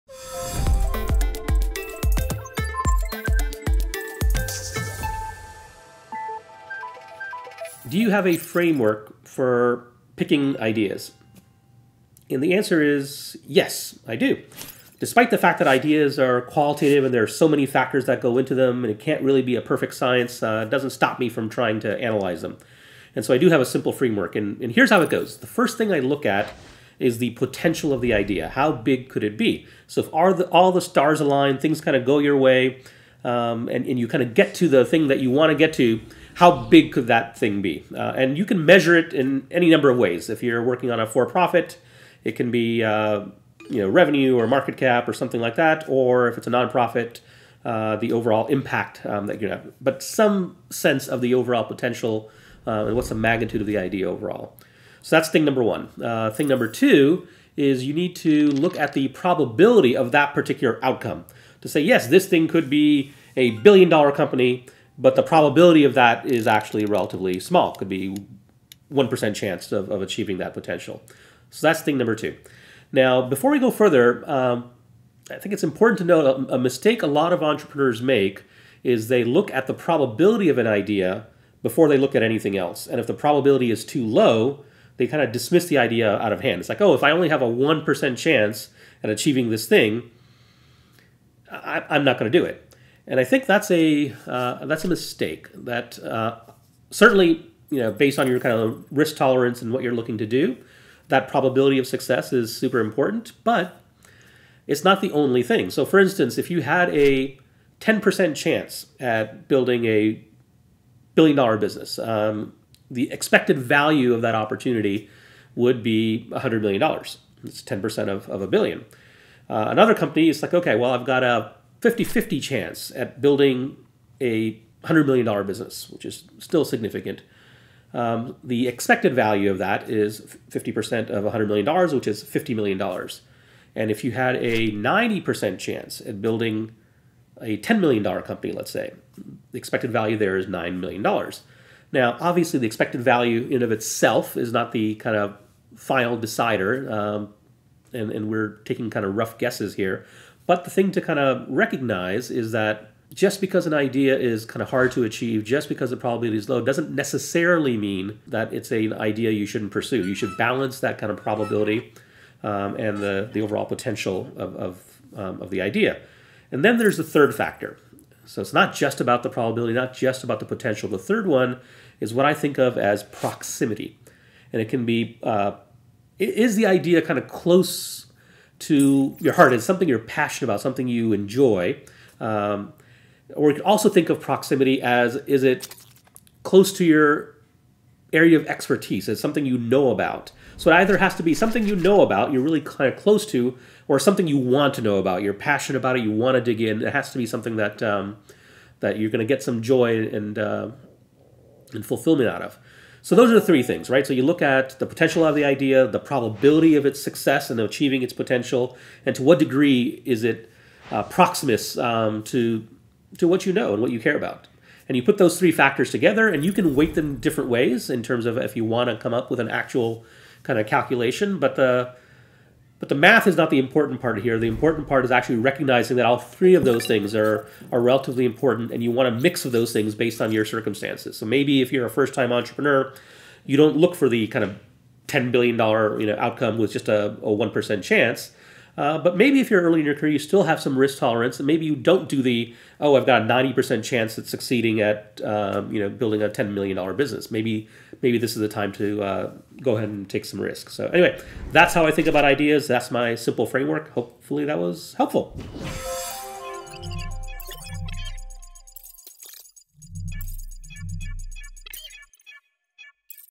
Simple Framework to Find the Best Startup Idea for YOU [Full Video]
Startup ideas are subjective, but that hasn’t stopped me from analyzing them and coming up with a framework to help you find the best startup idea for you.

This 3-part framework looks at the potential success of an idea (which is where most people go wrong) how likely you are to succeed, and how well it aligns with your passion or skills.

This will help you choose the right startup idea for you so you can avoid wasting time on startups that aren’t a good fit for you.